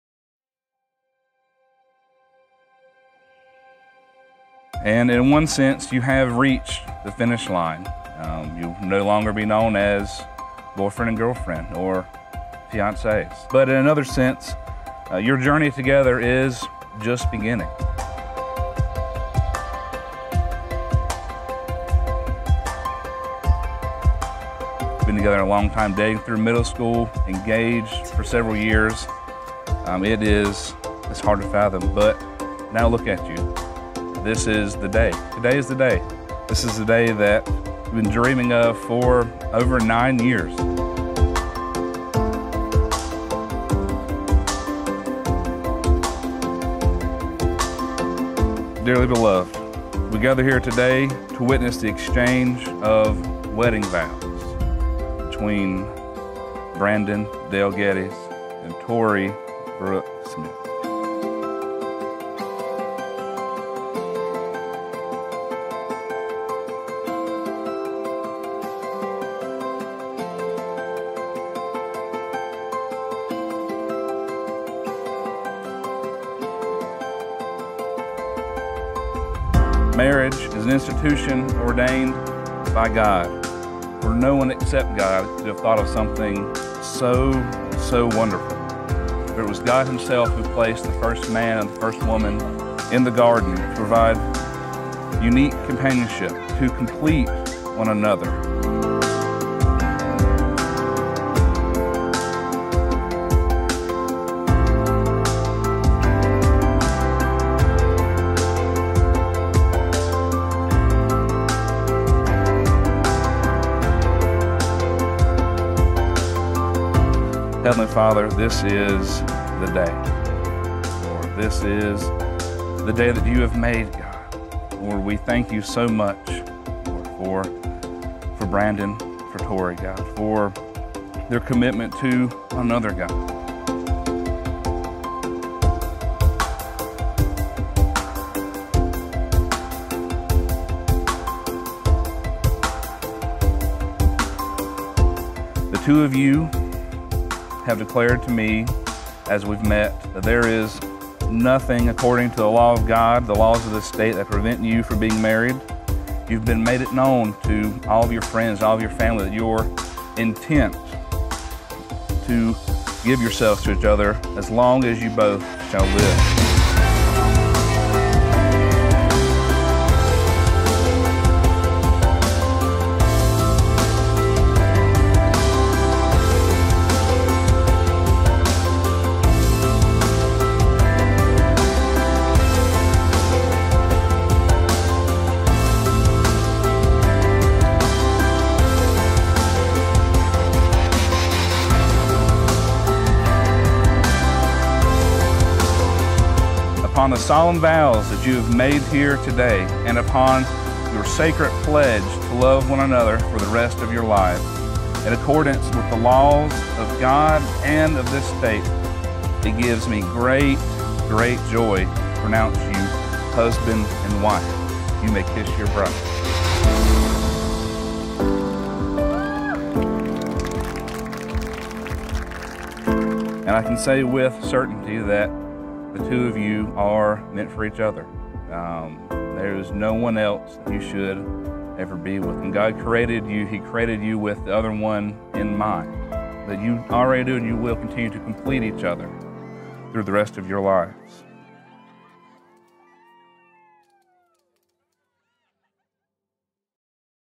Tori and Brandon
Tori + Brandon | we truly cannot put into words how special this day was to us. Every. Little. Detail. Absolutely perfect. We are so grateful to have captured both photo and video for such a stunning couple. Every vendor that day was literally commenting on how easy-going, bubbly and kind this couple was. We couldn’t agree more. Thank you so much for you love, kindness and trust Tori + Brandon! We wish y’all, your incredibly kind family and amazing bridal party the absolute best. Here’s to many, many years for the Gettys.

Venue: Lions Gate Manor
Coordinator: @eb_coordinating Emily Bishop Events
Photo/Video: Fipps Media
Florist: Mill Street Florist & Gift Shop
Cake: Carson's Creations
Catering: Holmes Catering and Events
MUA: Cassie Allie
Live Painter: Candler Reynolds
Dj: Palmetto Entertainment